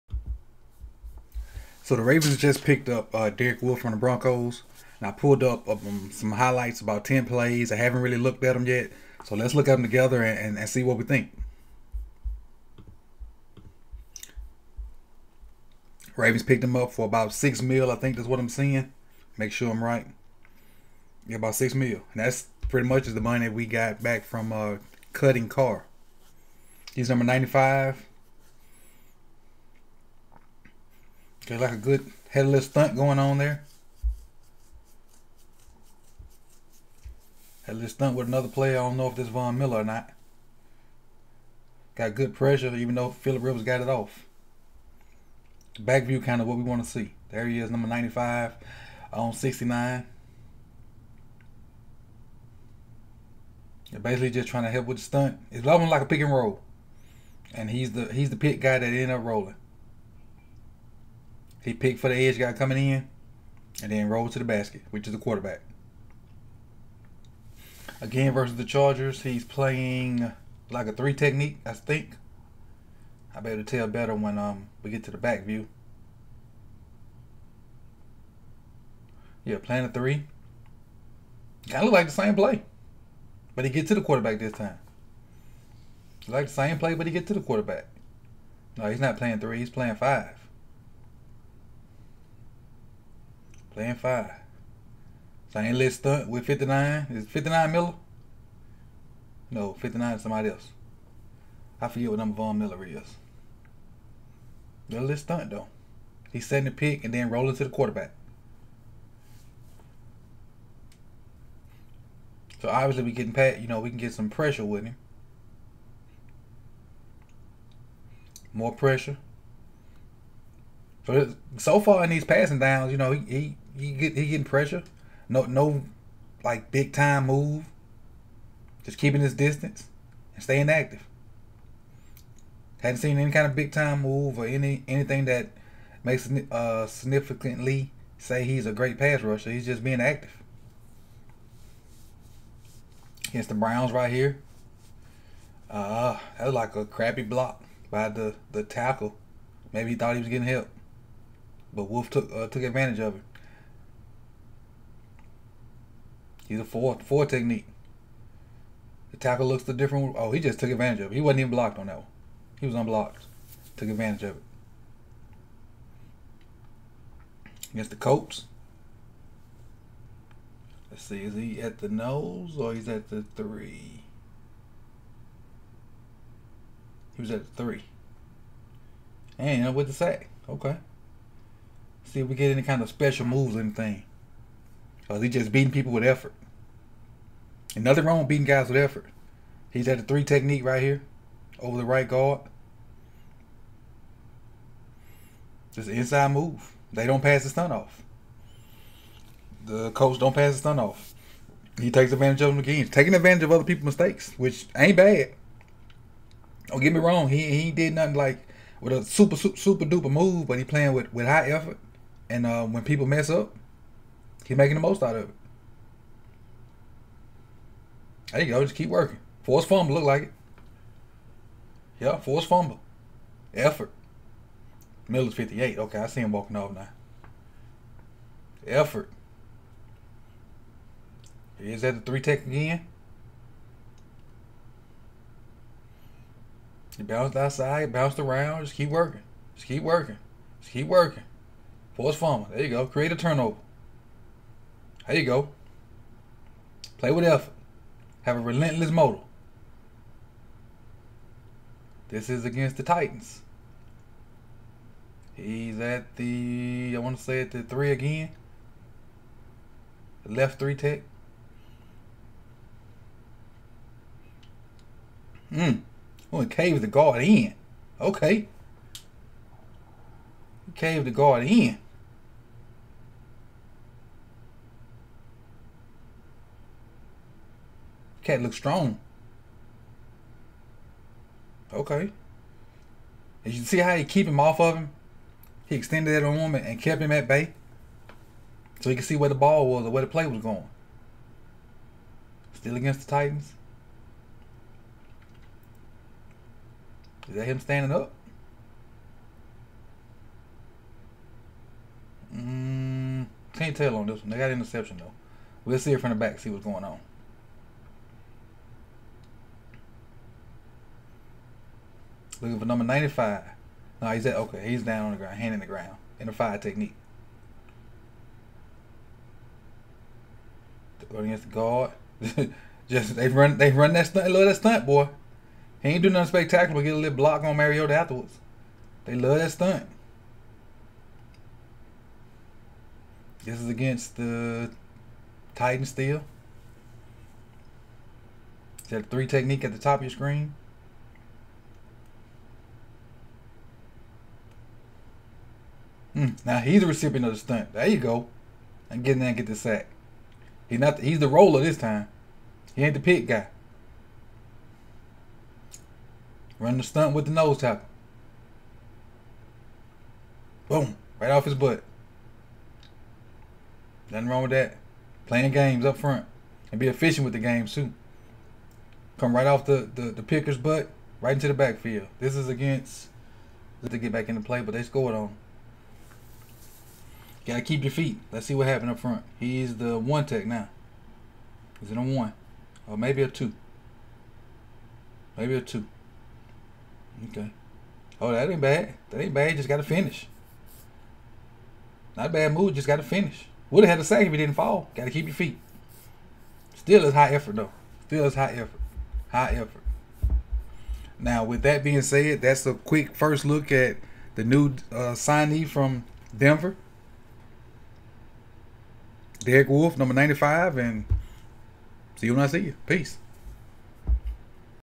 Welcome Derek Wolfe|| Baltimore Ravens Film Session
A breakdown of the Baltimore Ravens games. The good, bad, and ugly of the game.